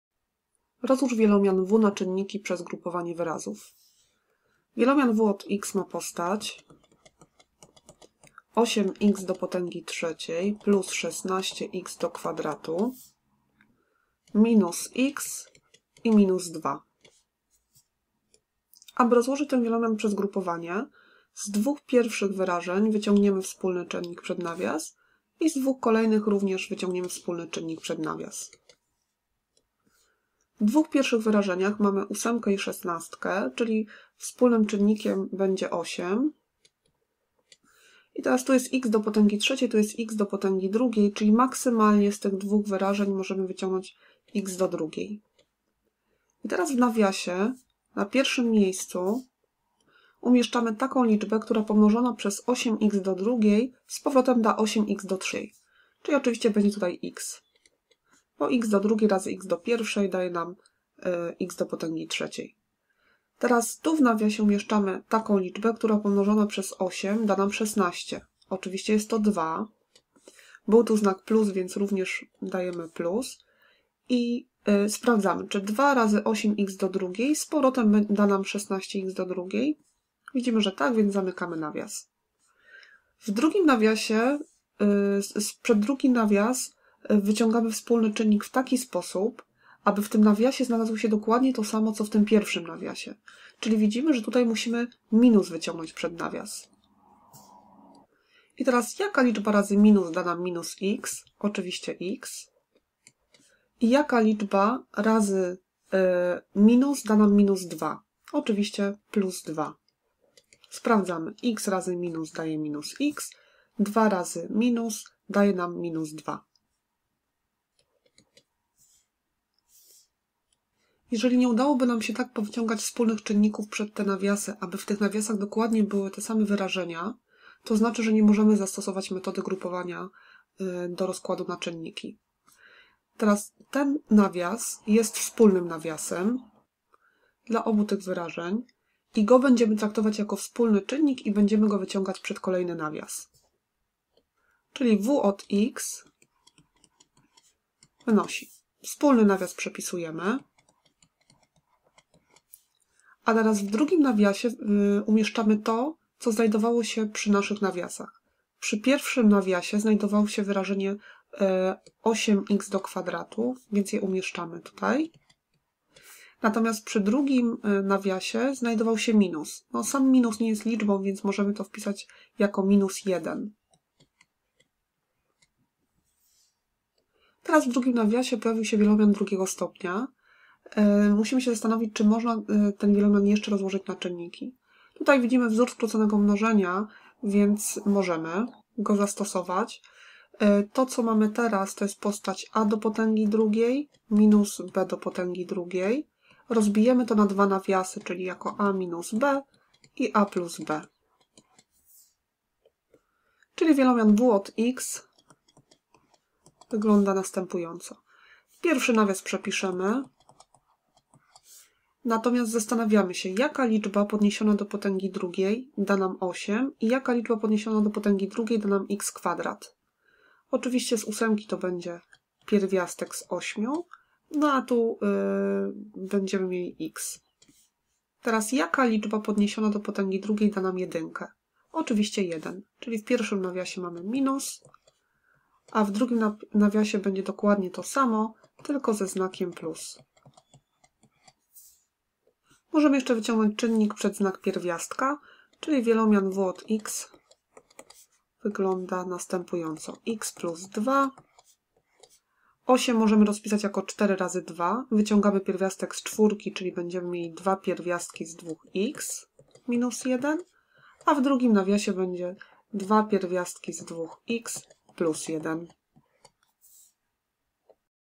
Rozkład wielomianu przez grupowanie wyrazów
Rozłóż wielomian 𝑤 na czynniki przez grupowanie wyrazów 𝑤(𝑥)=8𝑥^3+16𝑥^2−𝑥−2
Film powstał jako pomoc do kart pracy, które znajdują się na stronie w zakładce: MATMA: WIELOMIANY: ROZKŁAD WIELOMIANU NA CZYNNIKI.